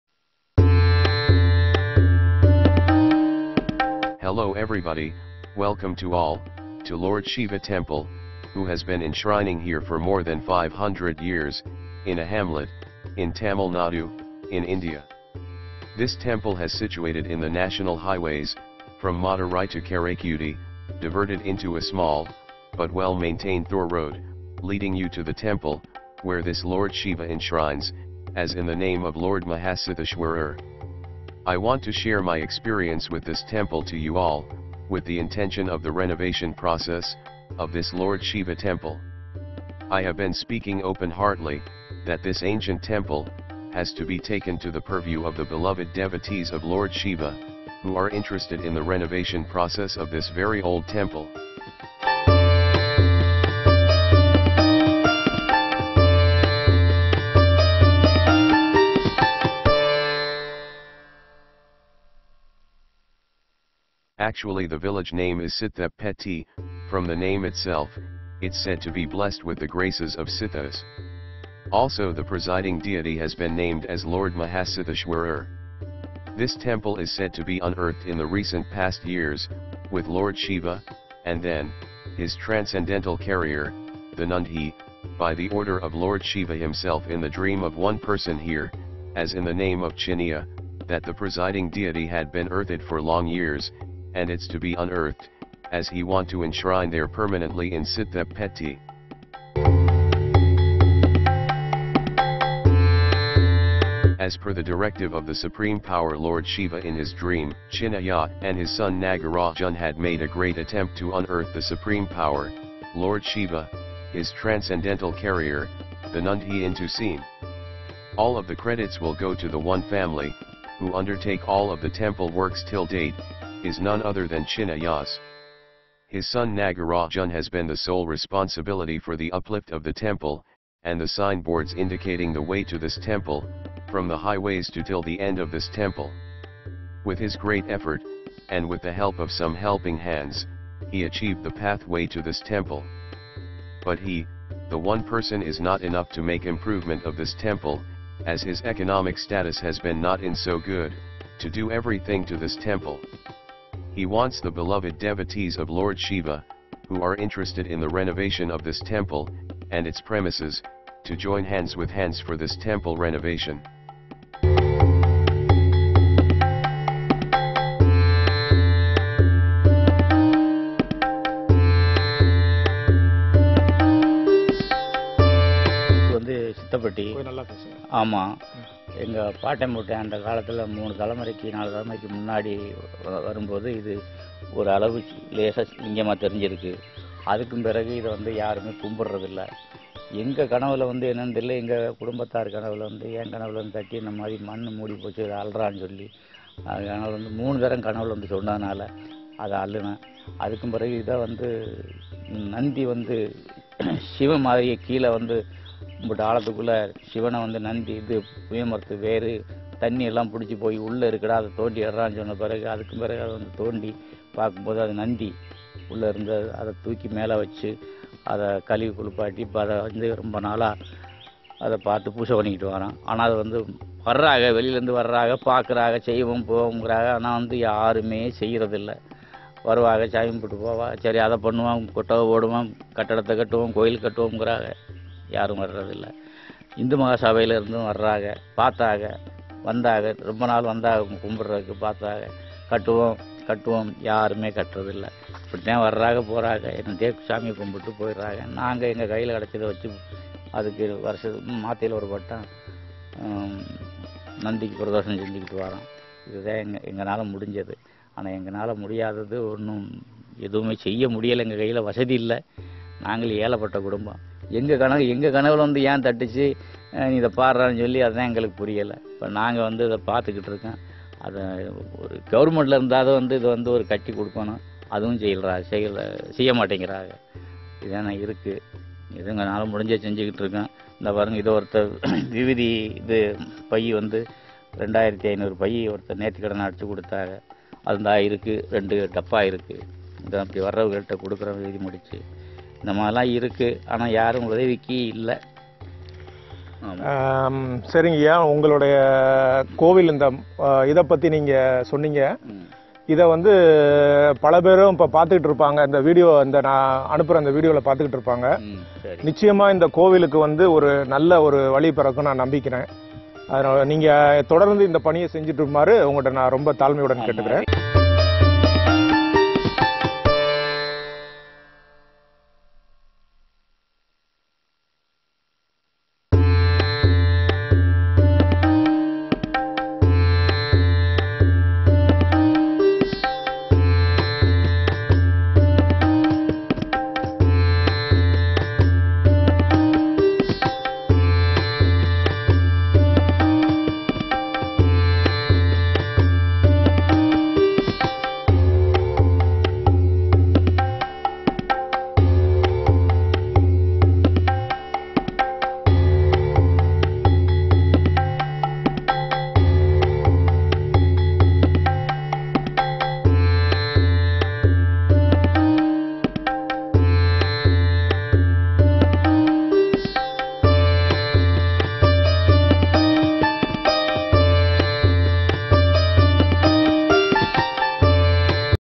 Sri Maha siddeshwarar ~ Near Thiruppathur/Karaikudi
Actually the village name is siddha patti, from the name itself, it's said to be blessed with the graces of siddhas. Also the presiding deity has been named as lord Maha Siddeshwarar. This temple is said to be unearthed in the recent past years, with Lord Shiva, and then, His transcendental carrier, the Nandhi, by the order of Lord shiva Himself in the dream of one person here, as in the name of Chinnaiah, that the presiding deity had been earthed for long years, and it's to be unearthed, as He want to enshrine there permanently in Siddha Patti.